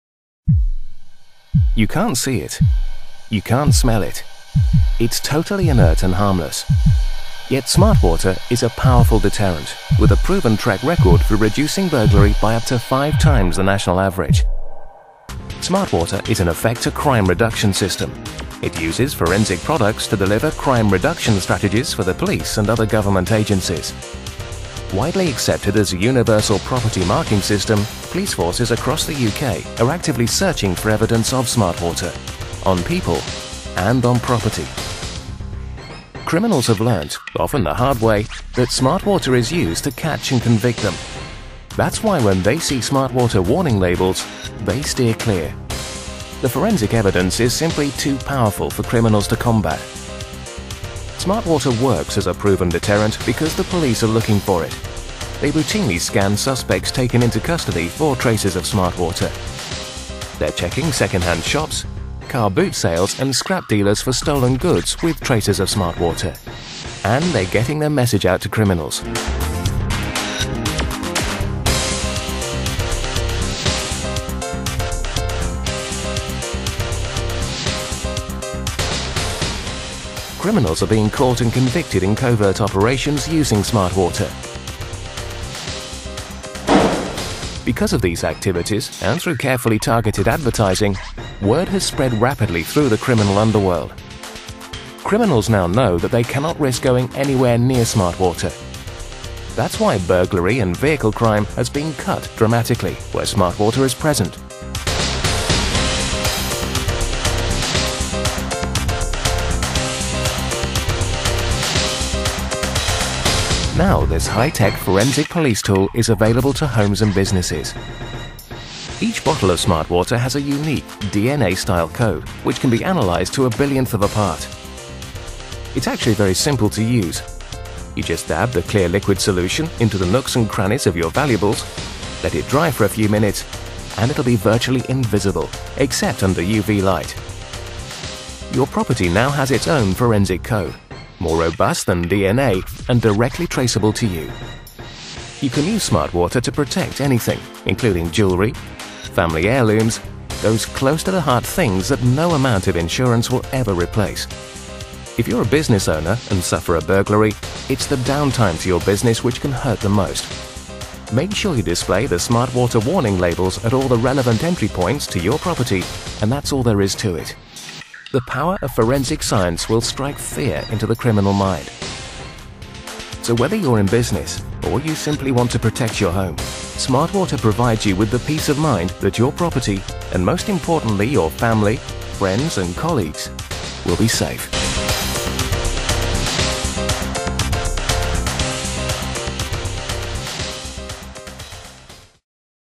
How SmartWater's forensic coding system works
Tougher than DNA, SmartWater is the only forensic coding system in the world that will withstand fire, humidity and bleach, SmartWater developed the SmartWater Strategy which, when working in partnership with the police, can produce a sustainable deterrent to crimes such as burglary, robbery and a range of environmental crimes.